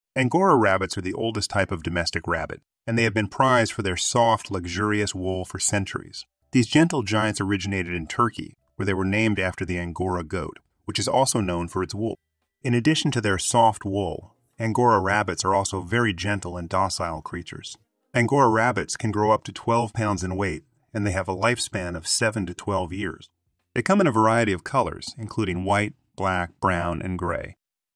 Angora Rabbits Are So Adorable 💕 They'll Melt Your Heart!nature
welcome to Nature's Notebook! In this episode we will unravel the interesting facts of🐰 Angora rabbit cutest😍 mammal I have even seen in my life. Did you know that this fascinating creature is originated in Turkey, and these creature are best producer of wool🧶, if not watch this video it will increase your knowledge about Angora rabbit.